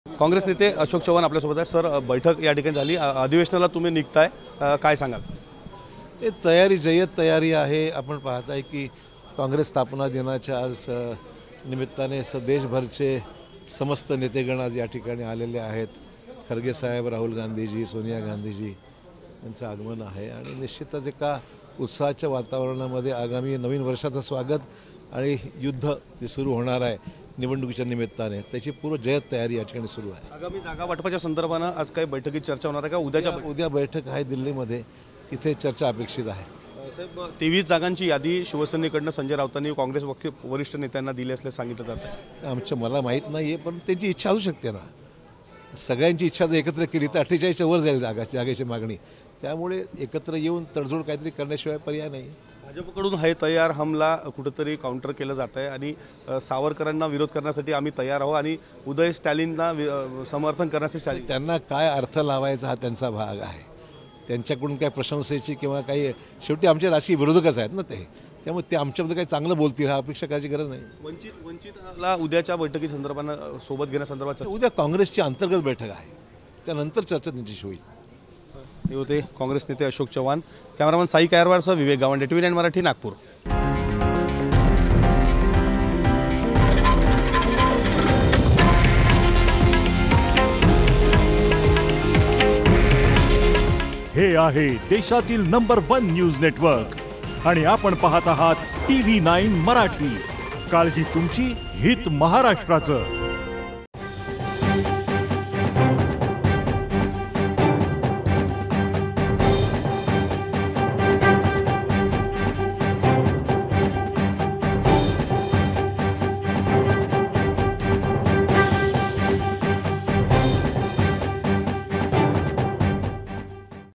लोकसभेसाठी महाविकास आघाडीच्या जागावाटपावर Ashok Chavan यांचं मोठं वक्तव्य
CREDIT - Political| Apeksha Sakpal /Producer|/Editor-DIGNATH/TV9D